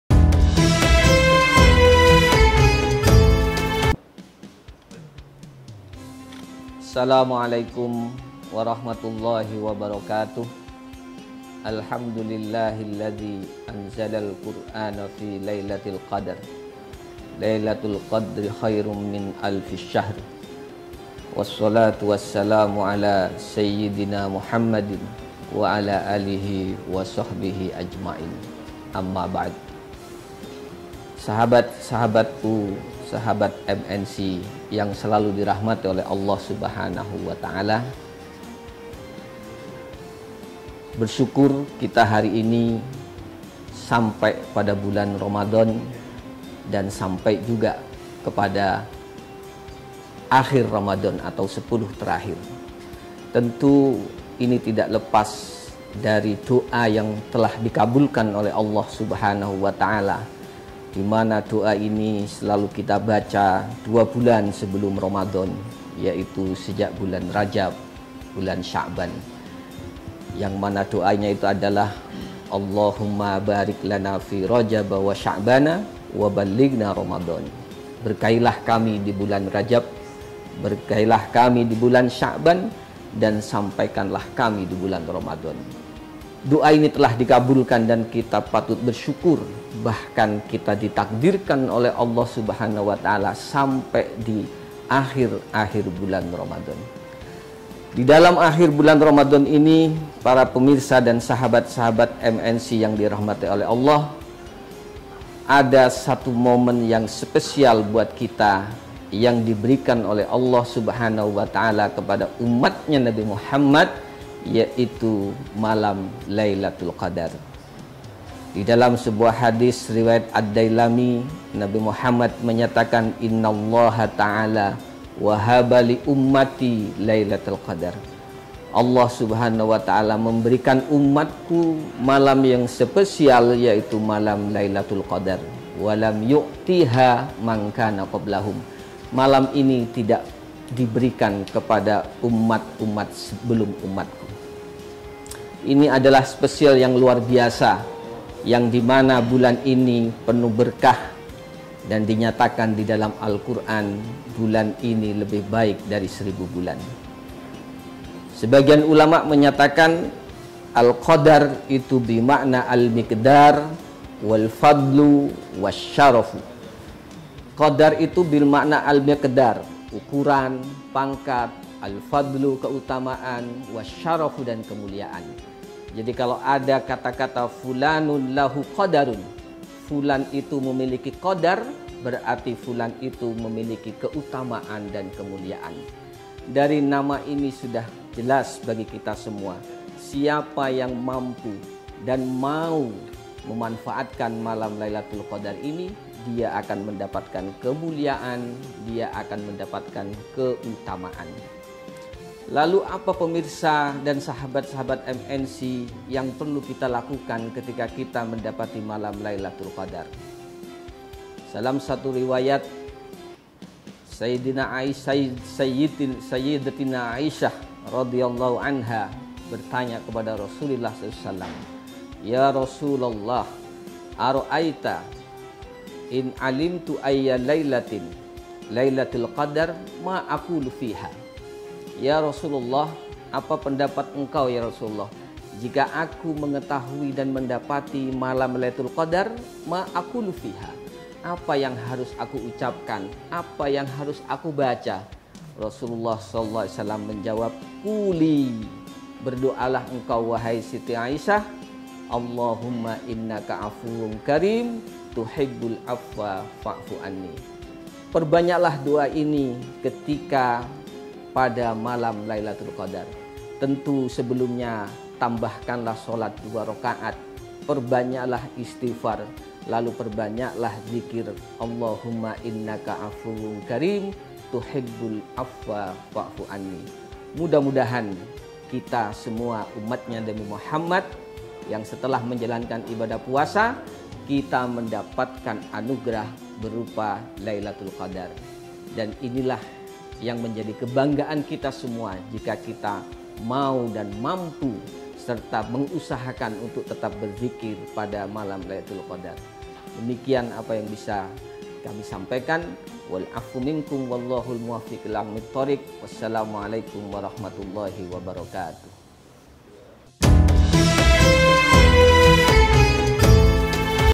Hikmah: Doa Lailatul Qadar
Kultum Hikmah bersama Ustaz Khoirul Anwar menceritakan tentang "Doa Lailatul Qadar".

HikmahiNews

Android: inews.id/android
iPhone: inews.id/ios